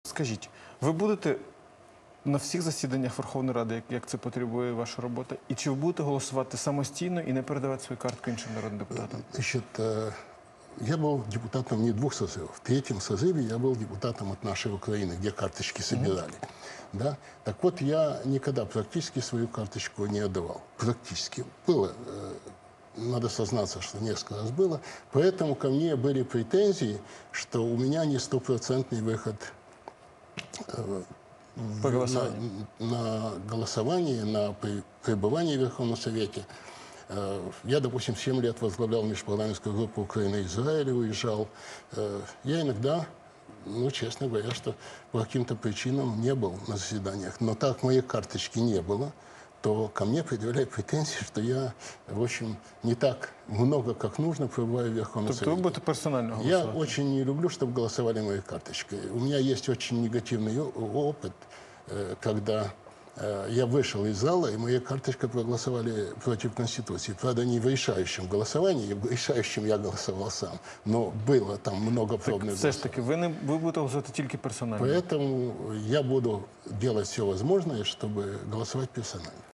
Гурвиц: я никогда своей карточки не отдавал
Эдуард Гурвиц в эфире ТВі